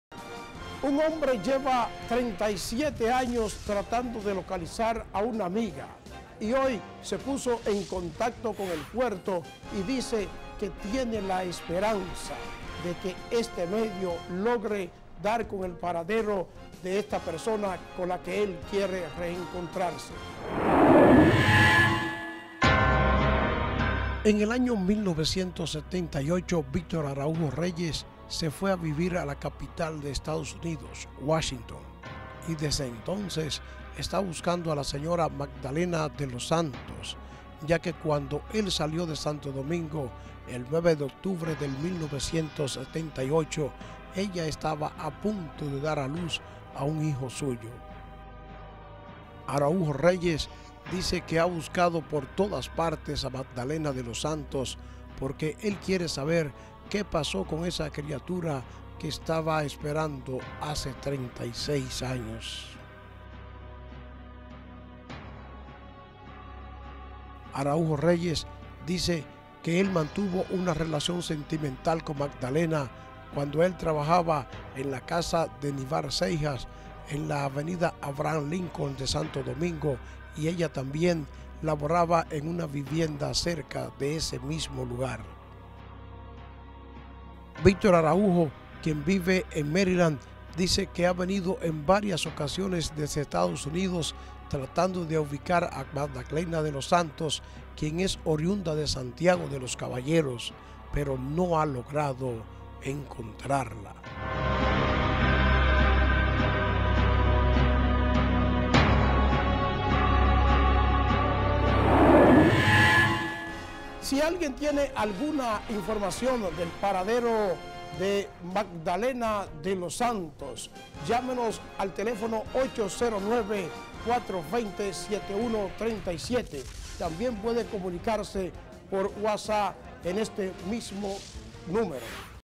VICTOR ARAUJO LLEVA 37 AÑOS BUSCANDO UNA MUJER
Desde Estados Unidos el señor Víctor Araujo se interesa por encontrar una mujer que conoció hace 37 años en Santo Domingo.